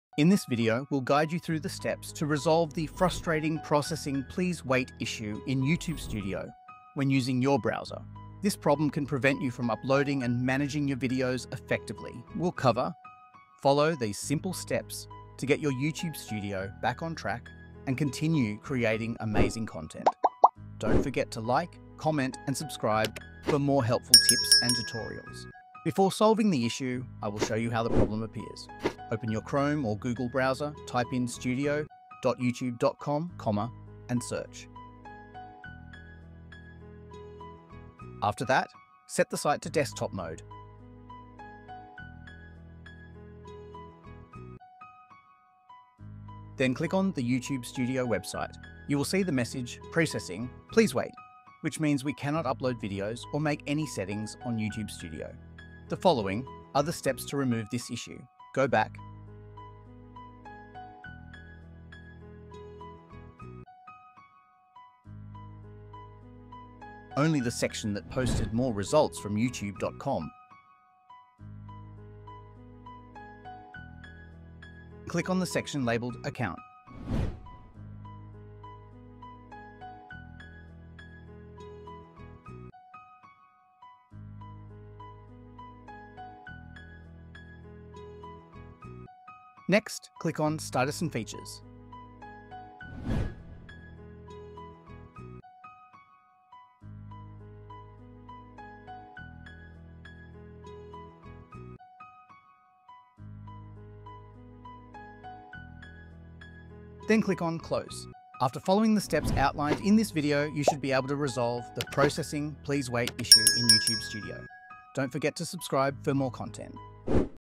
How to Fix "Processing. Please wait" Issue in YouTube Studio Browser
In this video, we'll guide you through the steps to resolve the frustrating "Processing. Please wait" issue in YouTube Studio when using your browser. This problem can prevent you from uploading and managing your videos effectively. We'll cover:

Follow these simple steps to get your YouTube Studio back on track and continue creating amazing content.

Before solving the issue, I will show you how the problem appears.

After following the steps outlined in this video, you should be able to resolve the "Processing. Please wait" issue in YouTube Studio.

My Query
YouTube Studio, processing issue, browser troubleshooting, YouTube upload problem, fix YouTube Studio errors